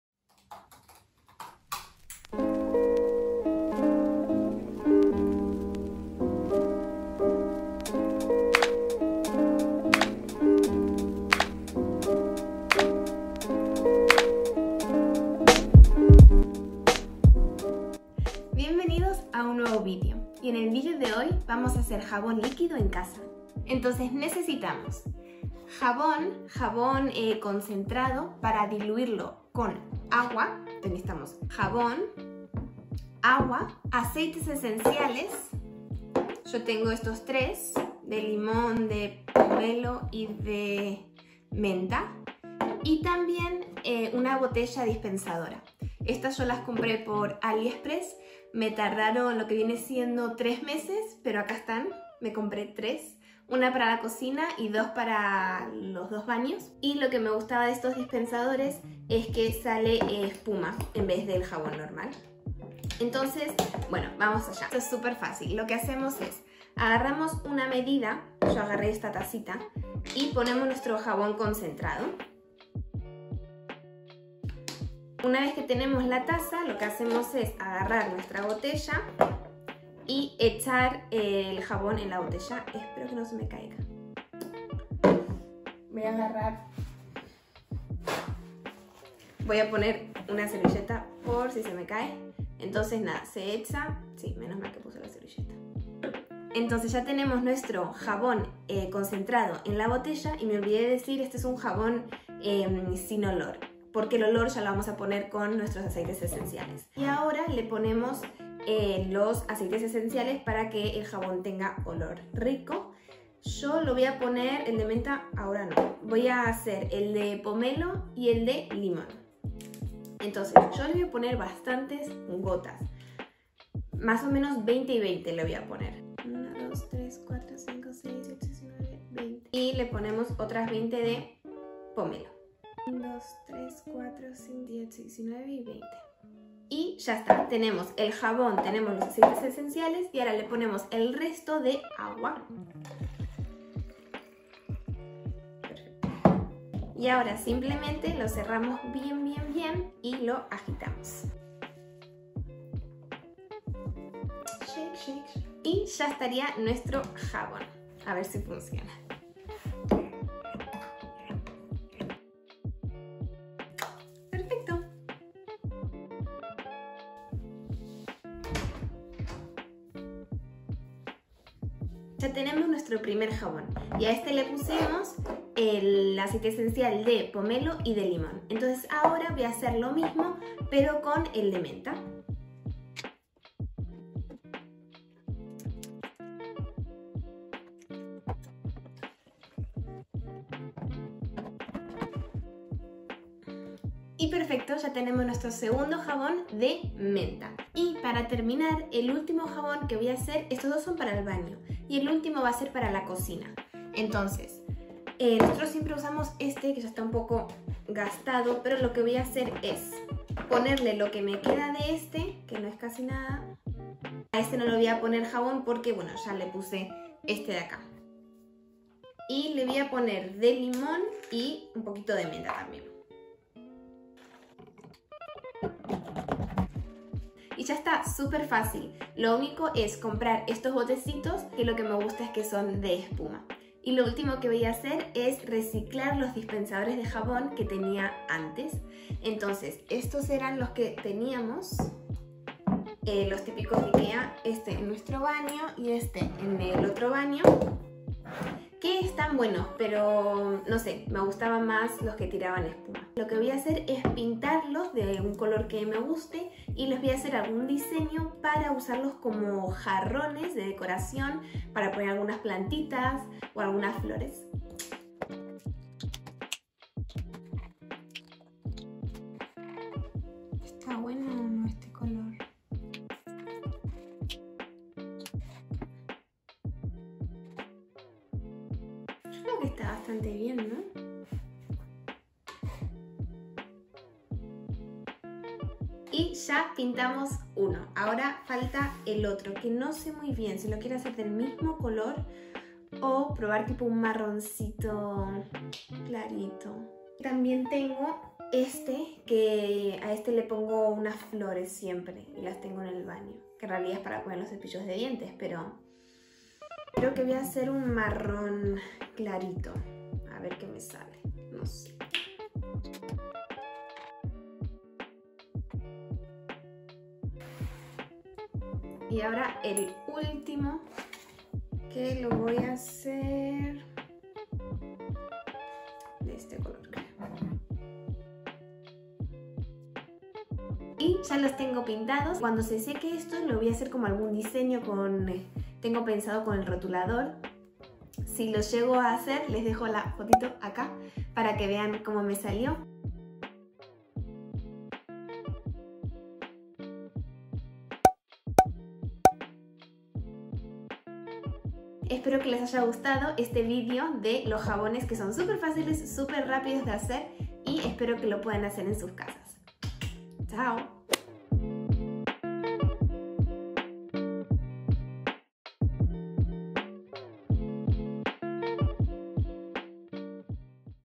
DIY JABÓN ESPUMOSO | + reciclo los dispensadores antiguos
En el vídeo de hoy te muestro cómo hacer tu propio JABÓN ESPUMOSO y reciclo los dispensadores que antes usaba, pintándolos y utilizándolos como decoración.

+ dispensadores - AliExpress

+ jabón: Dr. Bronner's Baby Unscented Pure - Castille Soap.

"Él perdona todos tus pecados y sana todas tus dolencias; él rescata tu vida del sepulcro y te cubre de amor y compasión; él colma de bienes tu vida y te rejuvenece como a las águilas."